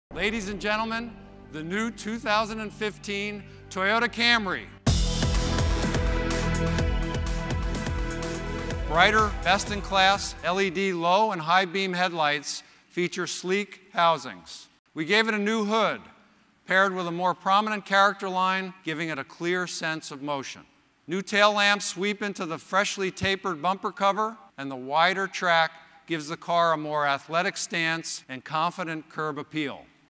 Exterior Features of the 2015 Toyota Camry
Learn about some of the exterior features that make up the new 2015 Camry.